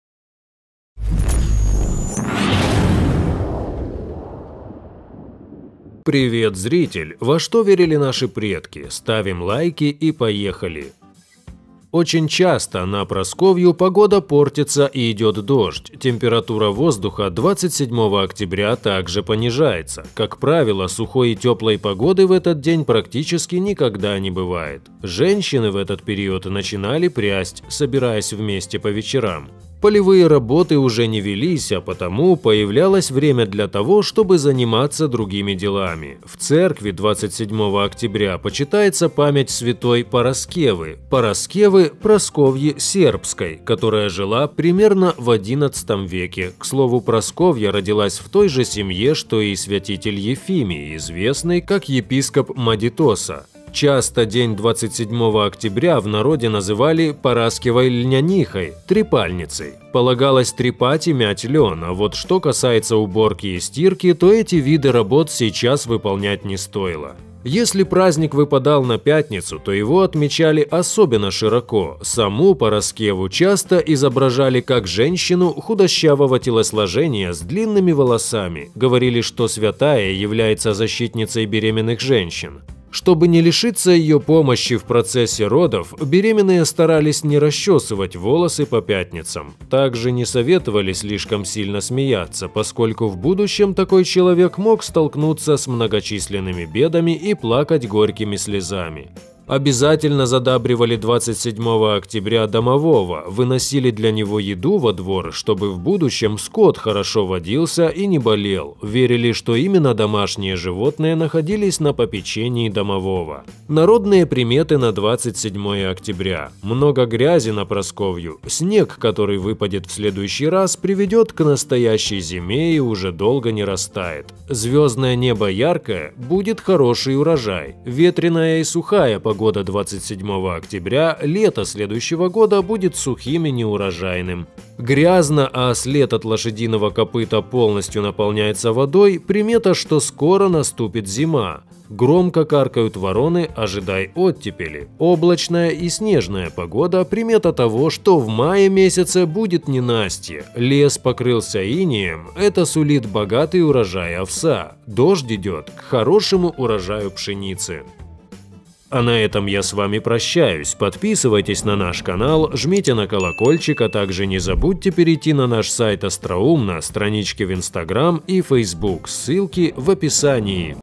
НАРОДНЫЕ ПРИМЕТЫ НА 27 ОКТЯБРЯ – ДЕНЬ ПРАСКОВЬИ
Во что верили наши предки?